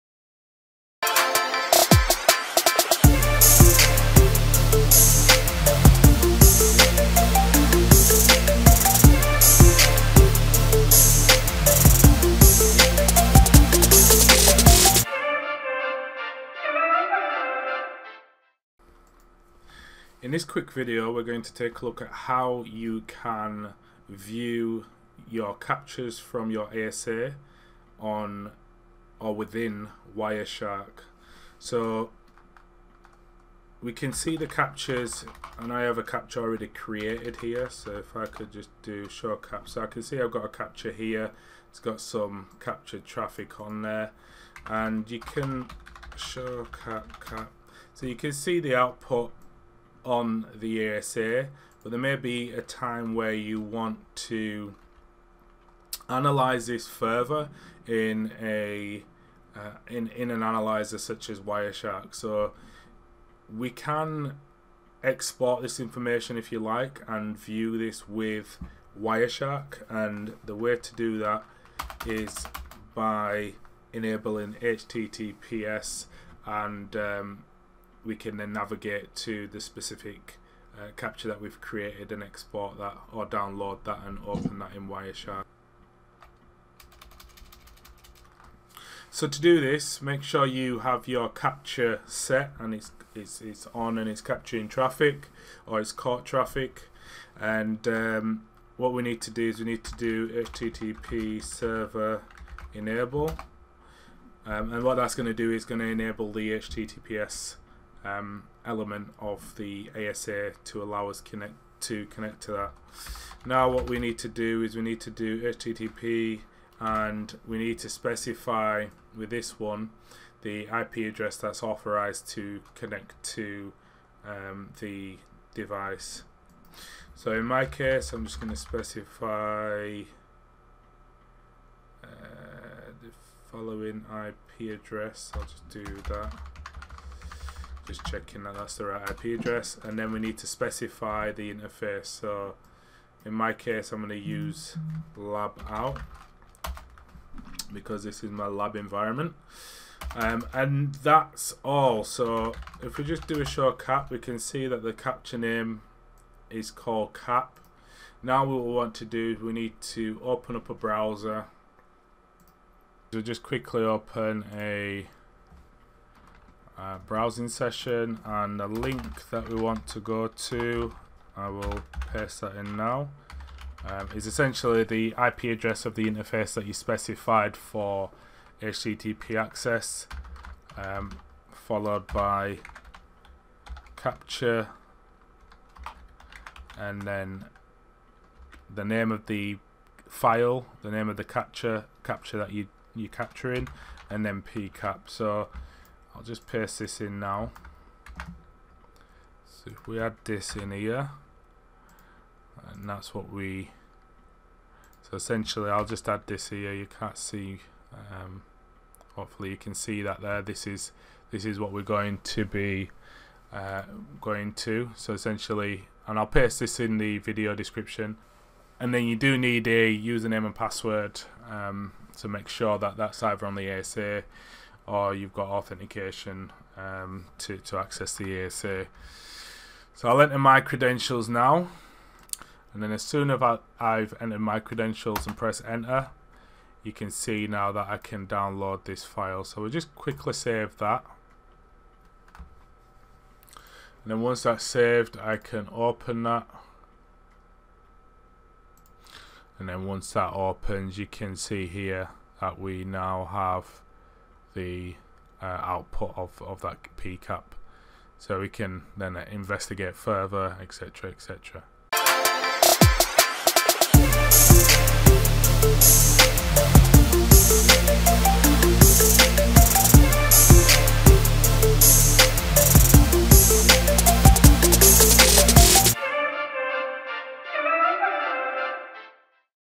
How to Export a Cisco ASA Packet Capture as a .PCAP | NWN18
In this video we take a look at how to export an ASA capture and use it in packet analysis tools such as Wireshark.

You can use the following link to access your devices capture file ip address]/capture/[capture name]/